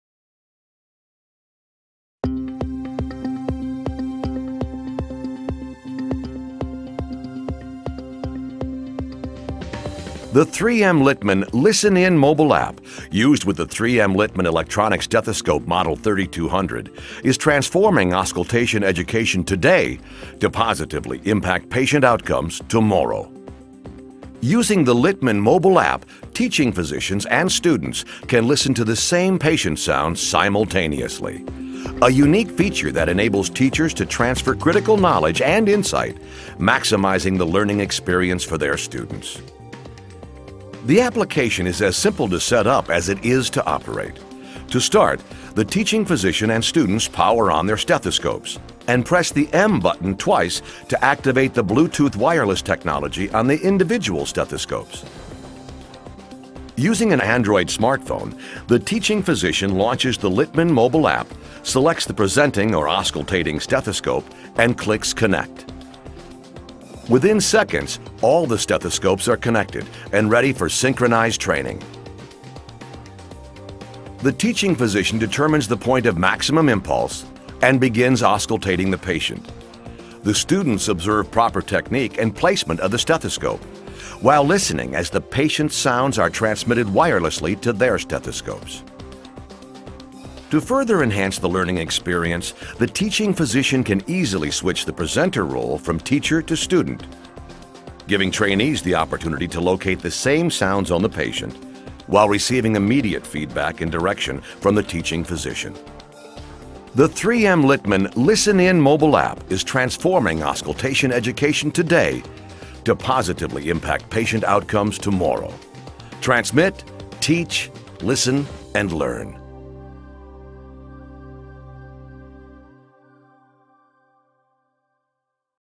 3M™ Littmann® Stethoscopes Listen-In Mobile Application
Utilizing the award-winning 3M™ Littmann® Electronic Stethoscope Model 3200 with
Bluetooth® technology, it is now possible for clinical educators to share a simultaneous
sound experience with up to four students.

The 3M™ Littmann® Listen-In Mobile Kit enables a mobile device to link with up
to five Littmann® Model 3200 stethoscopes. One stethoscope acts as the presenting
stethoscope while the others act as listening stethoscopes.

The 3M™ Littmann® Listen-In Mobile Kit includes five Littmann® Model 3200
Stethoscopes and a dedicated mobile device. Plug this technology into your
auscultation training program today.

Transforming auscultation education today -- to improve patient outcomes tomorrow.
• May reduce the length and increases the efficiency of teaching sessions
and rounds
• Presenting stethoscope can be easily switched to listening stethoscope,
enabling teacher to listen to student perform the auscultation
• Equivalent sound reproduction between one presenting stethoscope
and up to four listening stethoscopes
• Improves the confidence of both educator and trainee, because there is
certainty of the sounds that are being heard simultaneously
• Presenting stethoscope controls the filter (Bell, Diaphragm, Extended
Range) on all stethoscopes while each stethoscope has independent
control of the volume